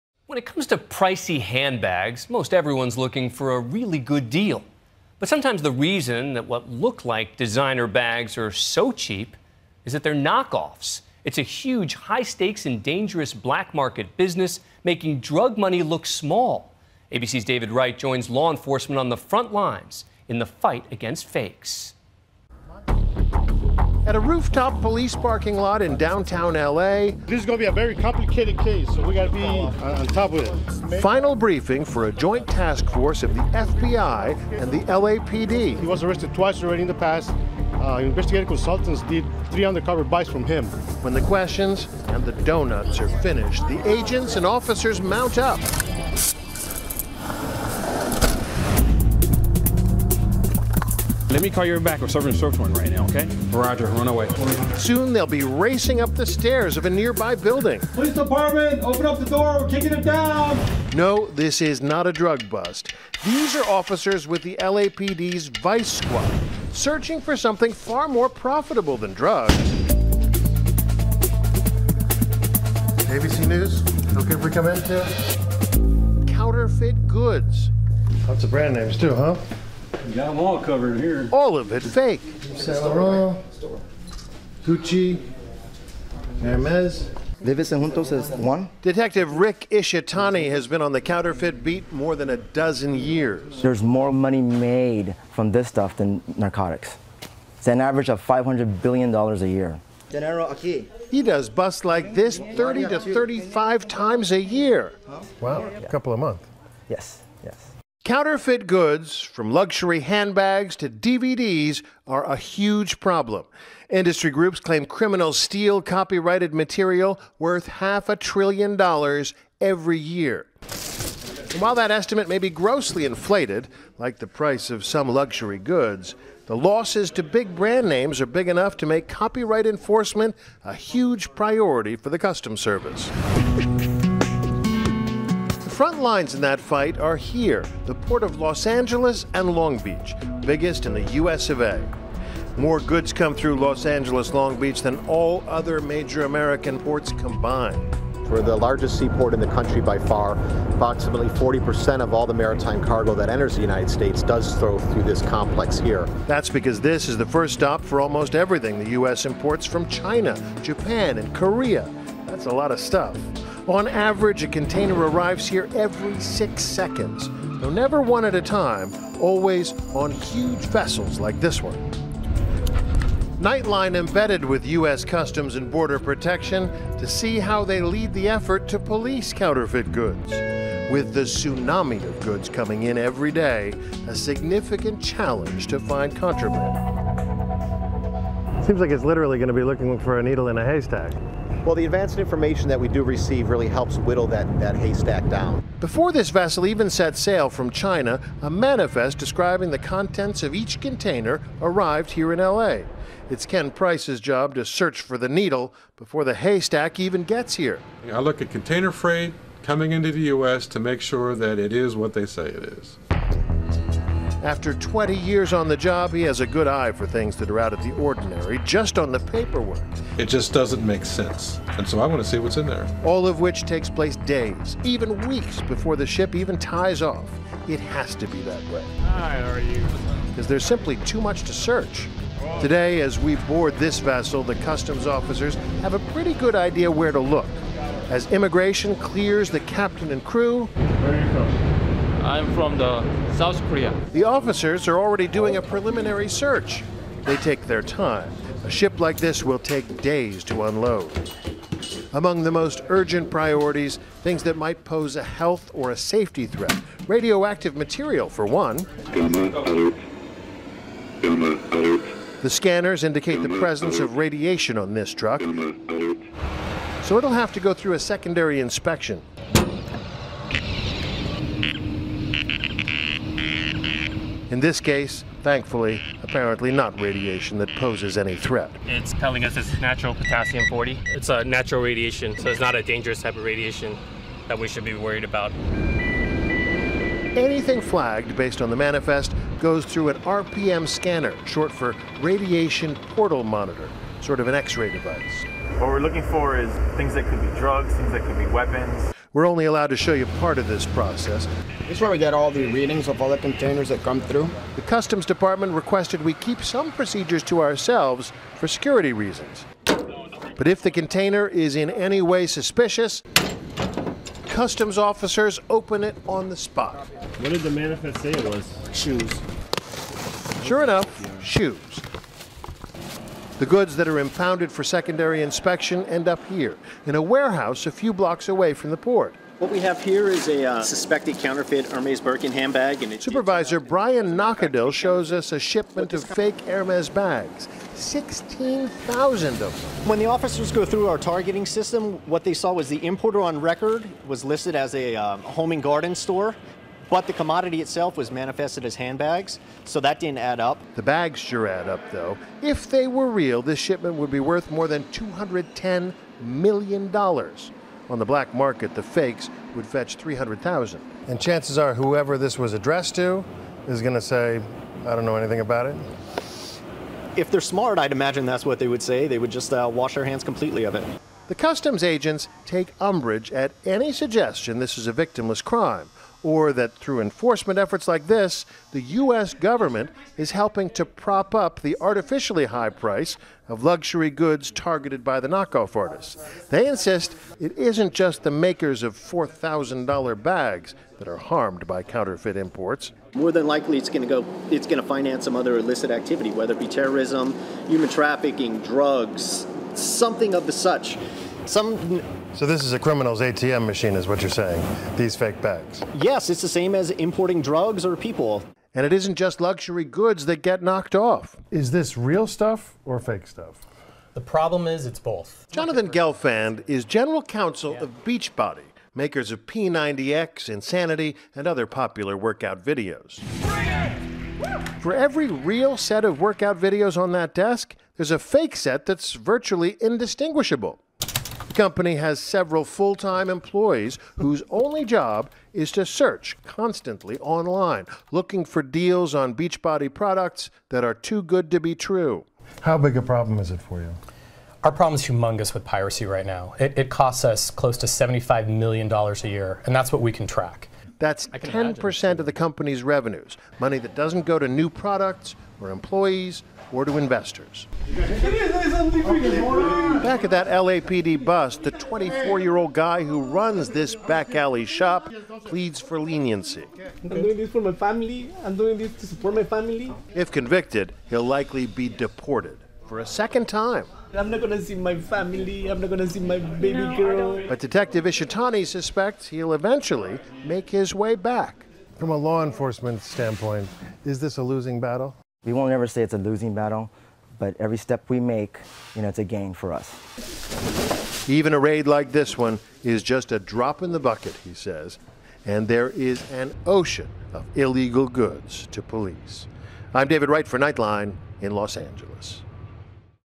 Los Angeles Authorities' Counterfeit Crusade
LAPD and U.S. Customs are fighting to try to stop a tidal wave of illegal imports.